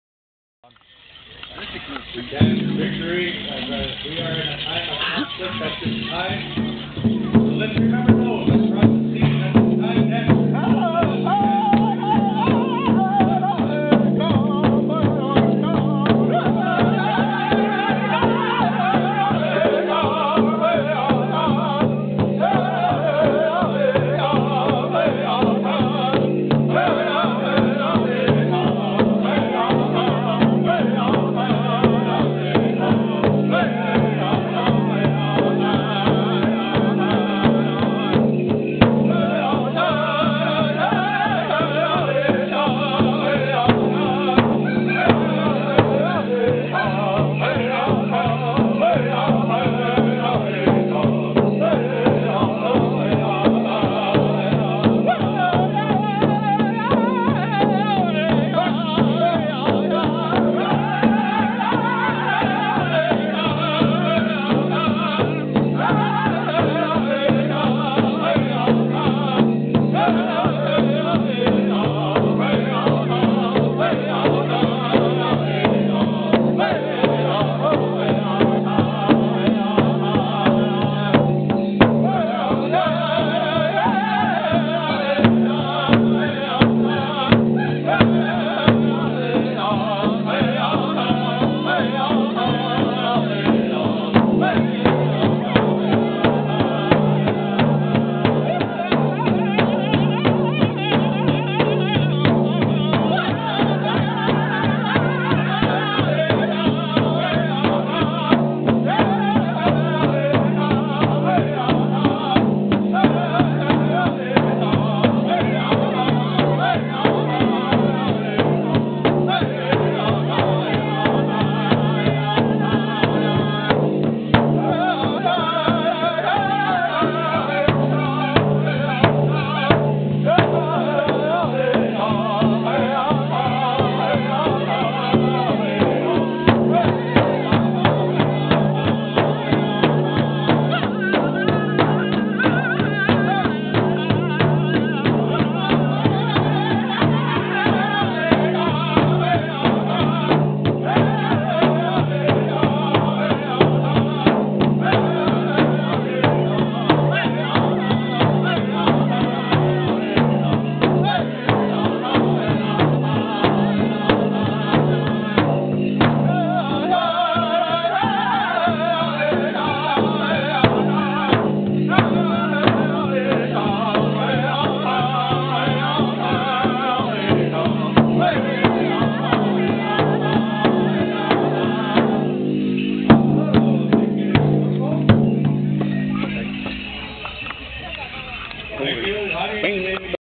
Roy Track Memorial Mesa Pow Wow 2008- Short Bull.
Short Bull singin' the victory song after Sunday grand entry.